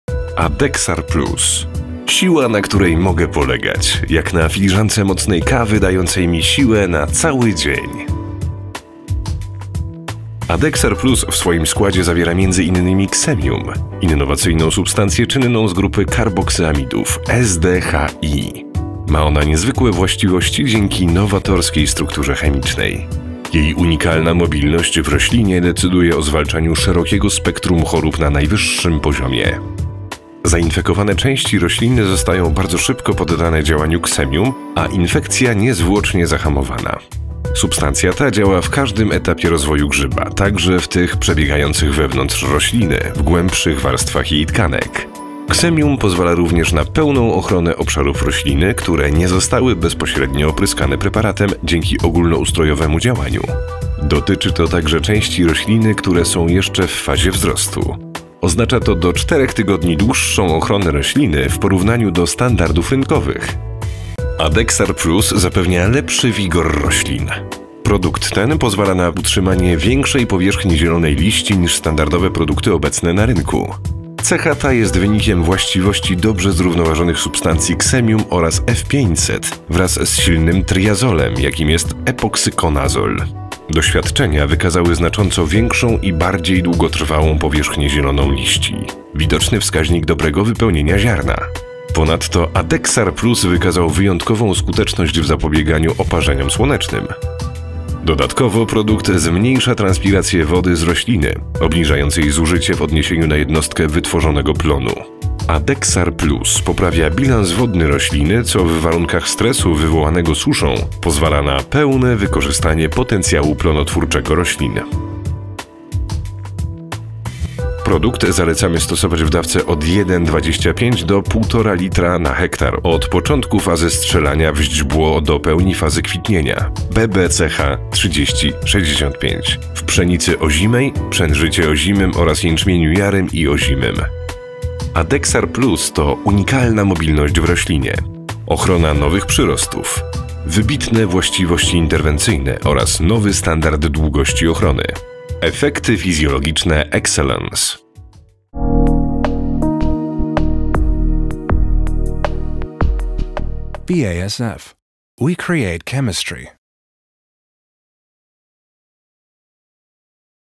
Adexar Plus z Xemium - siła fungicydu zbożowego na której można polegać
Adexar Plus z nowoczesną substancje aktywną Xemium został uznany za innowację BASF roku 2013. Xemium® – to innowacyjna substancja czynna posiadającaz niezwykłe właściwości dzięki nowatorskiej strukturze chemicznej.